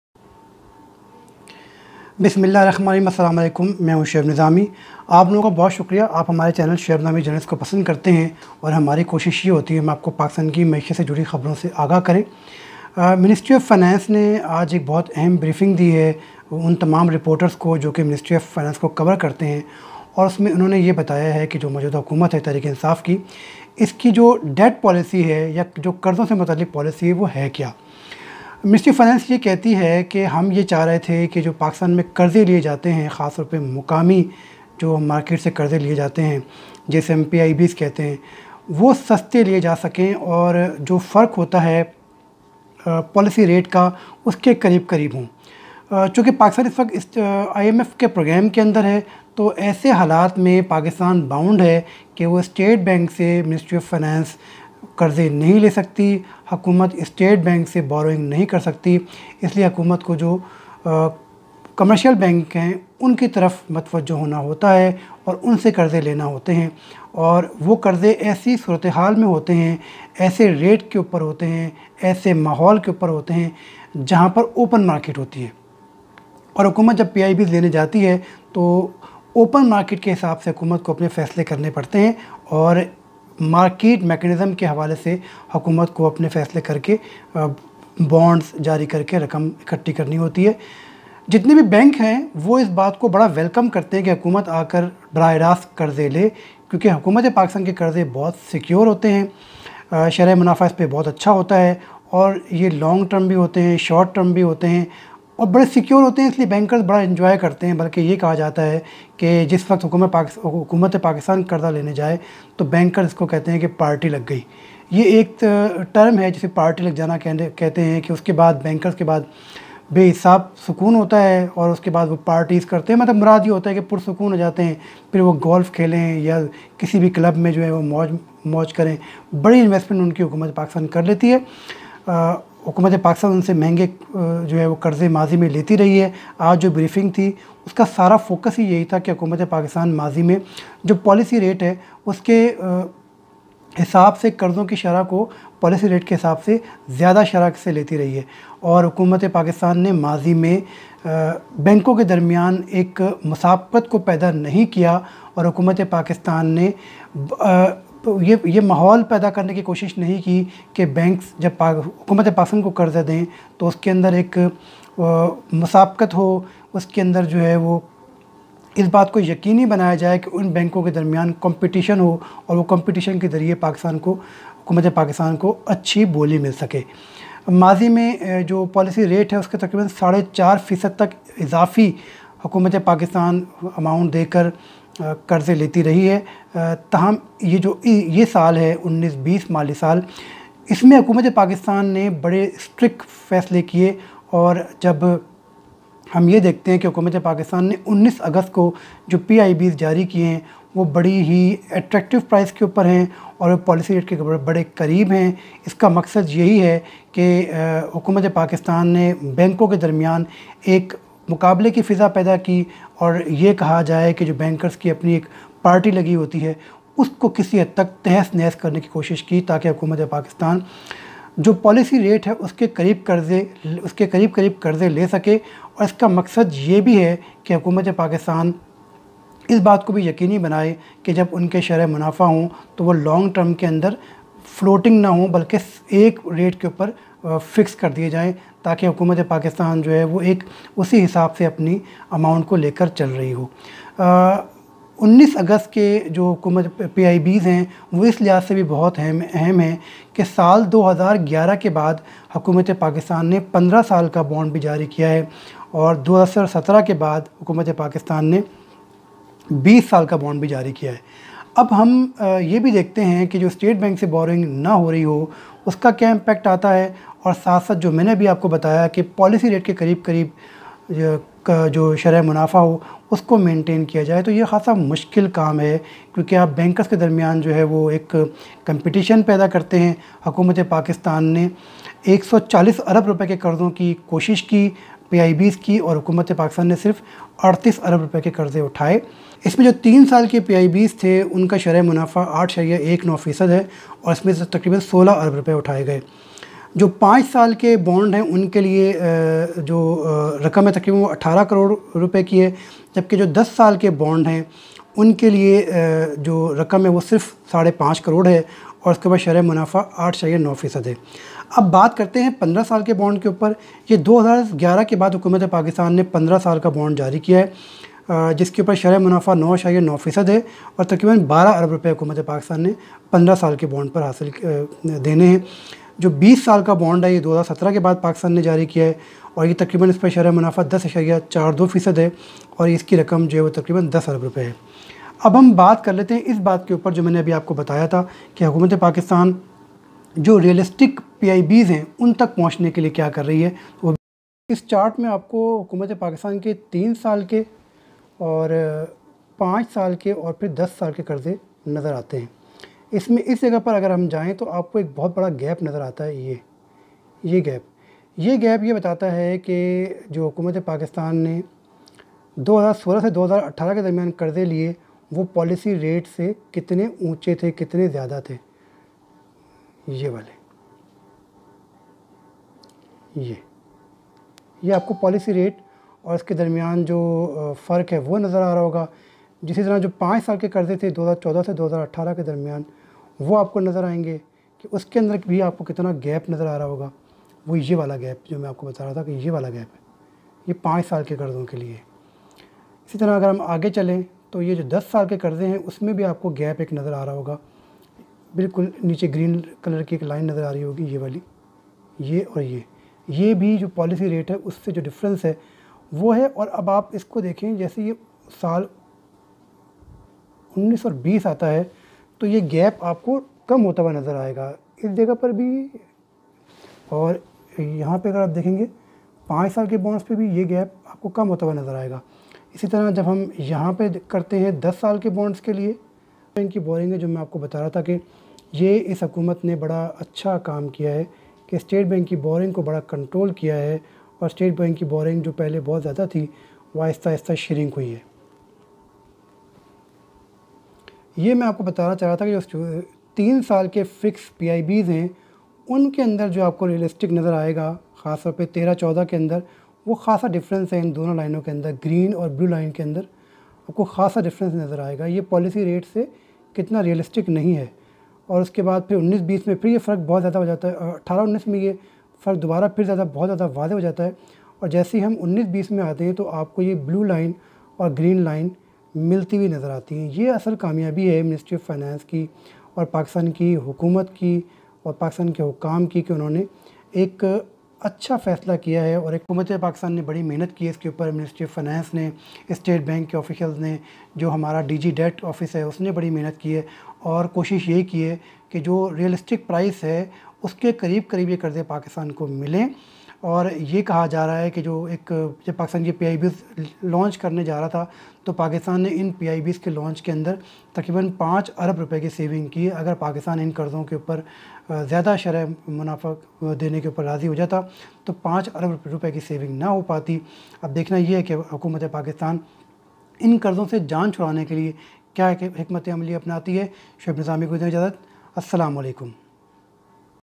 Finance Ministry Debt Policy
The debt policy coordination and management office showed improvement in four out of five debt indicators.
The average time of maturity of the total domestic debt increased from 1.5 years to 4.1 years.
Even after excluding the debt re-profiling, the maturity period increased to 2.7 years.
However, the average time of maturity of the external debt decreased from 7.5 years to 7.1 years due to the government’s reliance on short-term foreign commercial loans.

pti official
pti uk
pti Lahore
pti Karachi
pti Islamabad
pti faisalabad
pti Peshawar
pti Abbottabad